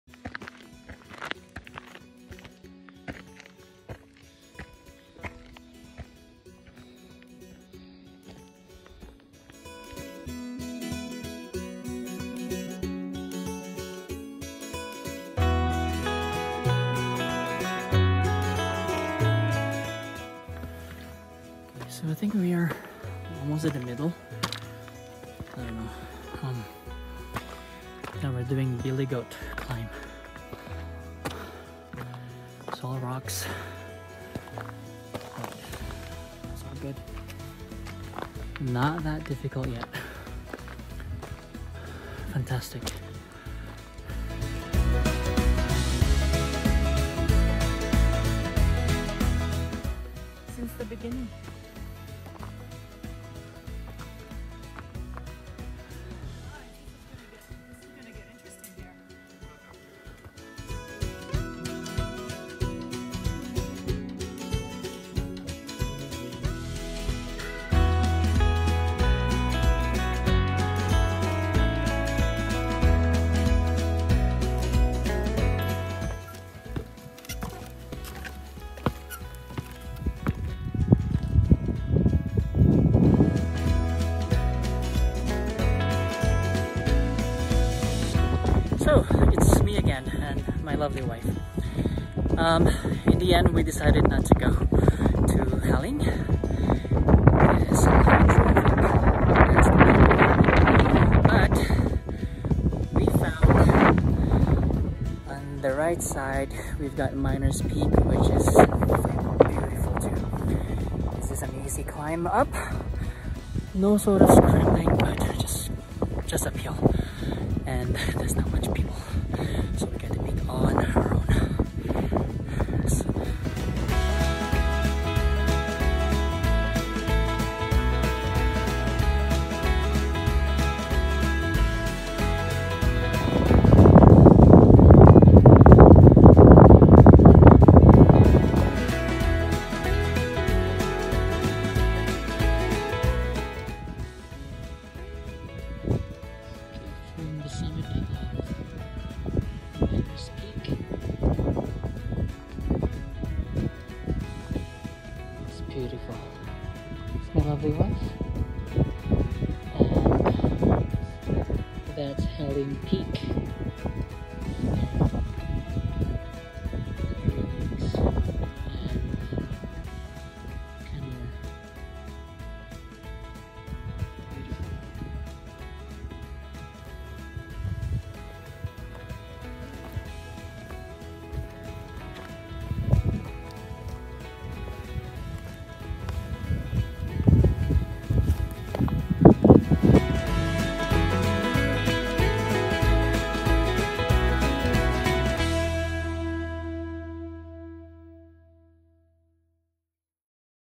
Miners Peak Via Ha ling
Miners peak and Ha ling shares the same trail. It is well maintained trail and lots of staircase going up.

A good hike today after 5 weeks of recovery from my grade 3 AC separation.